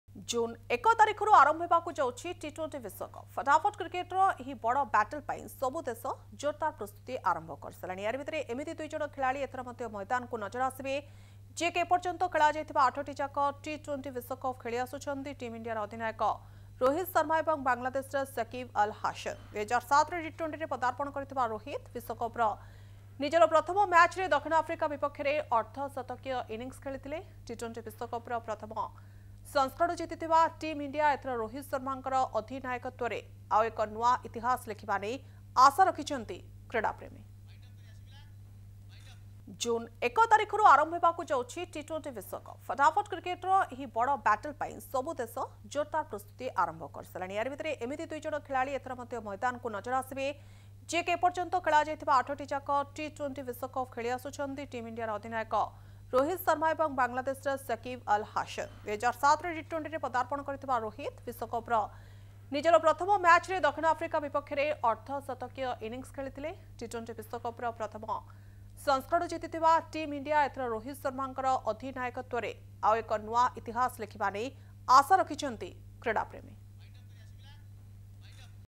Rohit Sharma & Shakib Al Hasan Set To Extend Remarkable Streak In ICC Men's T20 World Cup
Rohit Sharma and Shakib Al Hasan as the only two players to feature in every edition of the ICC Men’s T20 World Cup. With the tournament set to commence next month